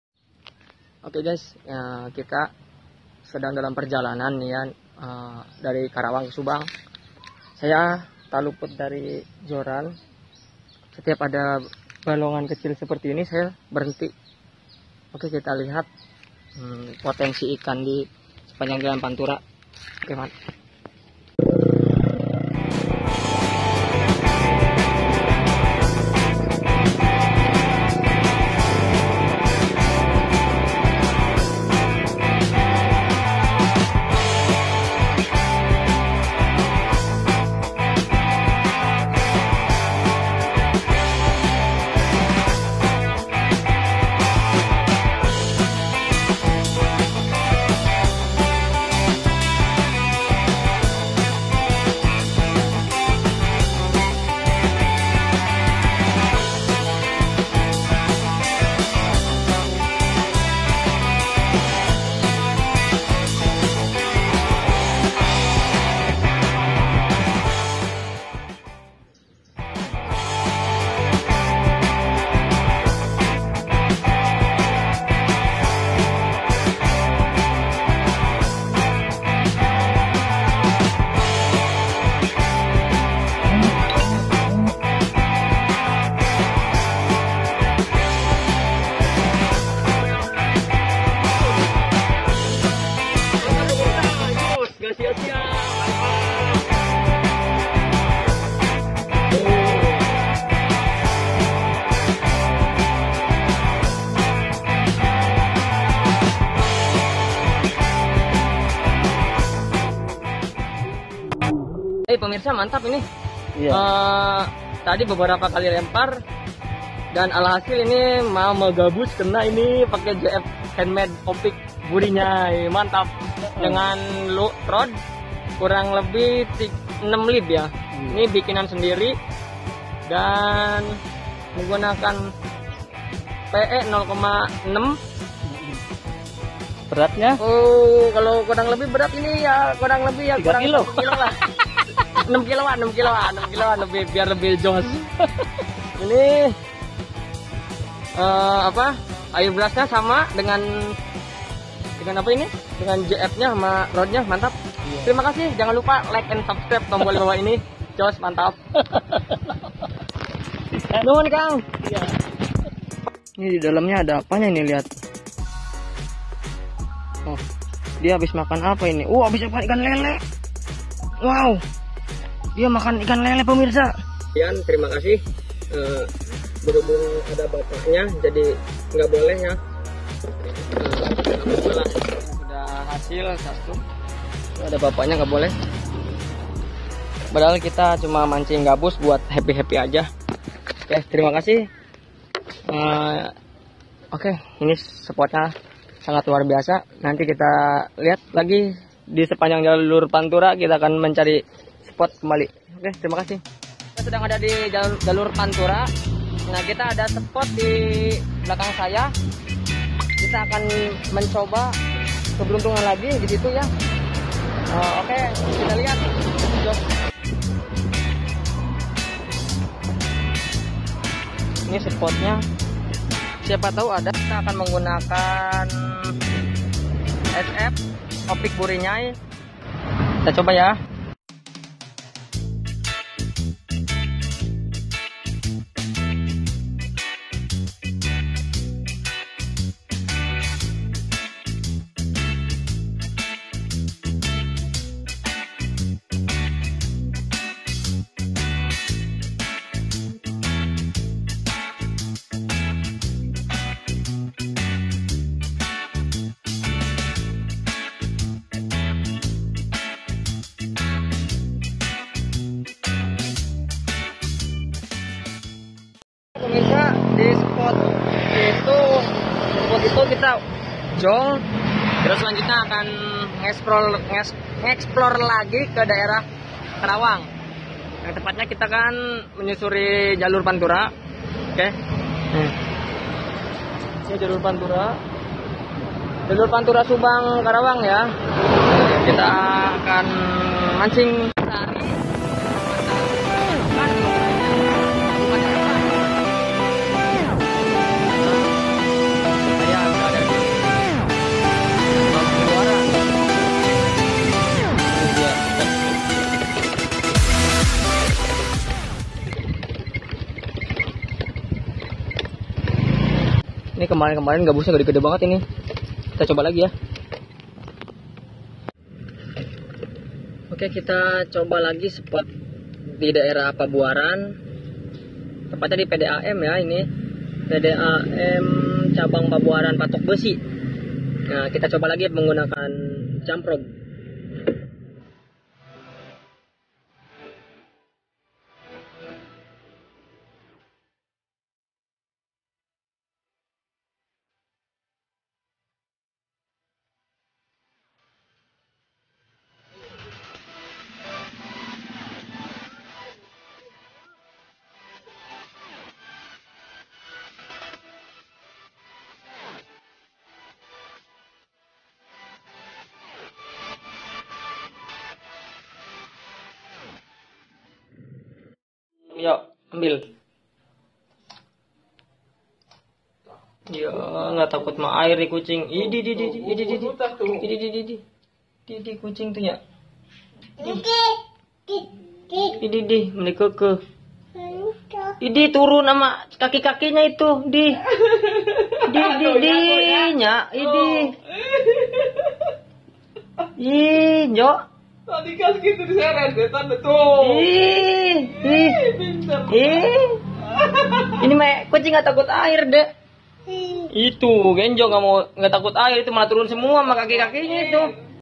Spot potensial, sepanjang jalur pantura subang_karawang Ultralight fishing
Sport potensial di jalur Pantura yang tepatnya di daerah munduSari Kecamatan pusakanagara Kabupaten Subang dan di daerah patokbeusi tepatnya di kecamatan Pabuaran samping PDAM situ dulu ada eceng gondok sama mata lele potensi Strike  luar biasa di situ.